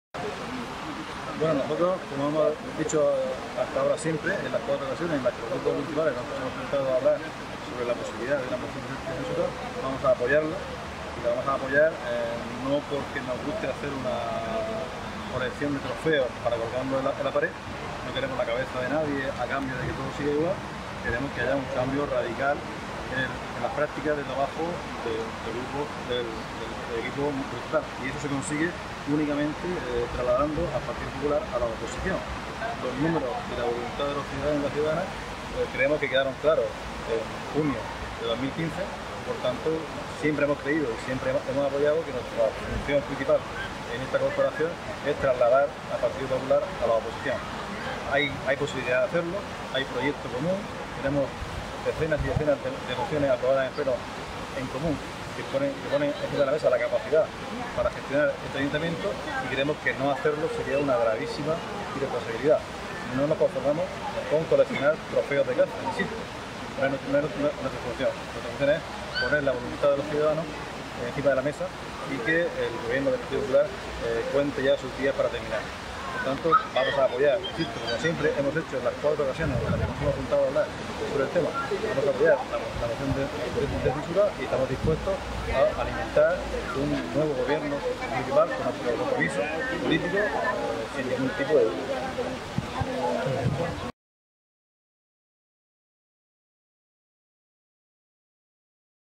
Moción de censura
Una regeneración democrática real, como esa de la que tanto se hace gala Ciudadanos, no se consigue entregando la cabeza de un concejal a cambio de que todo siga igual, se consigue cambiando de gobierno.